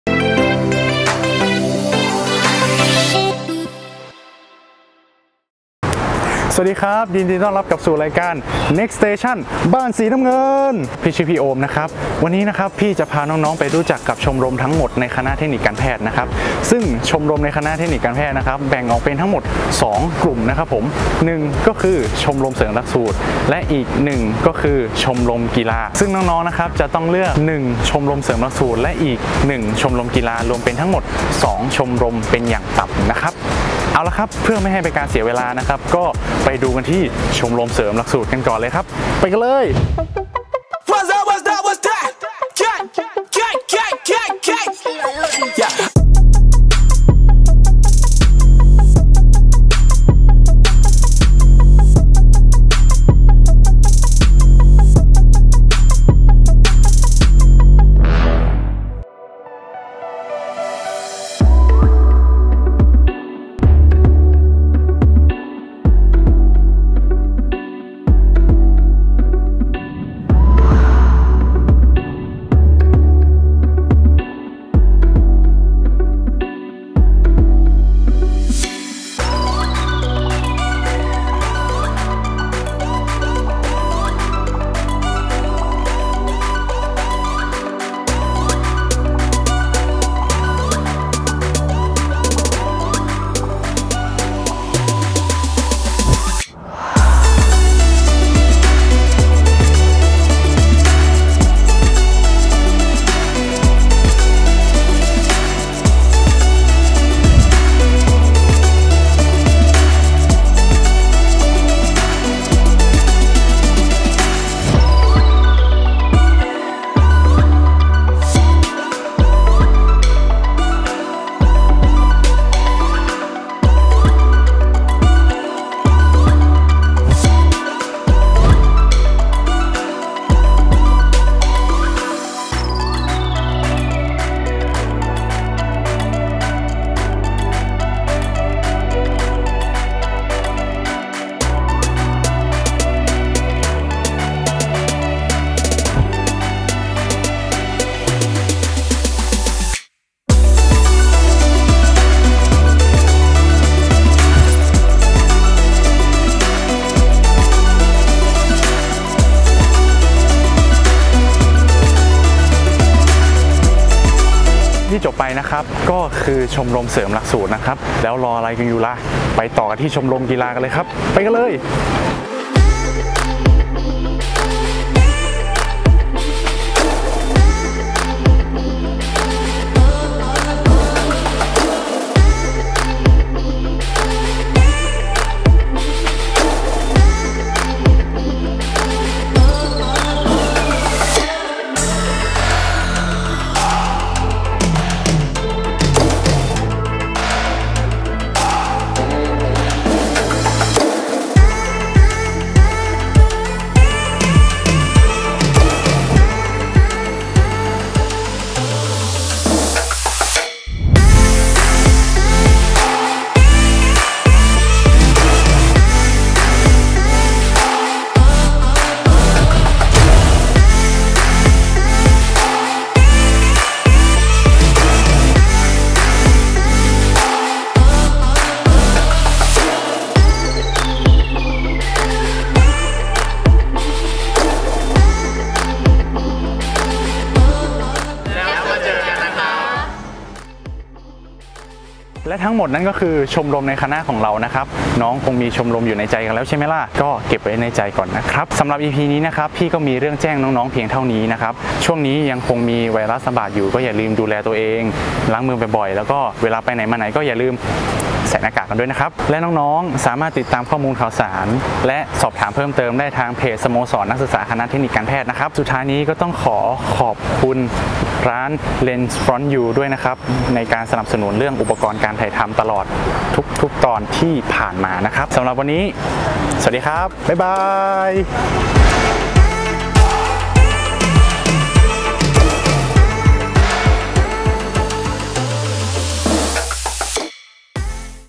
Ep.5 | ชมรมในคณะเทคนิคการแพทย์ มช.
“ แจวเรือไปซื้อขนม แจวเรือไปซื้อขนม...
ชวนมาเข้าชมรม ร่วมกันแจววๆ ! 🚣‍♀️🚣‍♀️🚣‍♀️ “
รายการ Next station...บ้านสีน้ำเงิน  🏡💙
มาชวนน้องๆ ให้ได้รู้จักกับชมรมต่างๆที่น่าสนใจ ในคณะเทคนิคการแพทย์  ไม่ว่าจะเป็น
🌷 ชมรมเสริมหลักสูตร
🌷 ชมรมกีฬา
ซึ่งแต่ละชมรมก็มีกิจกรรมมากมายให้น้องๆได้ร่วมสนุก พัฒนาทักษะนอกห้องเรียน แล้วมาร่วมสนุกกันน้าาาาา
 💙 สามารถติดตามข่าวสารและกิจกรรมต่างๆได้ที่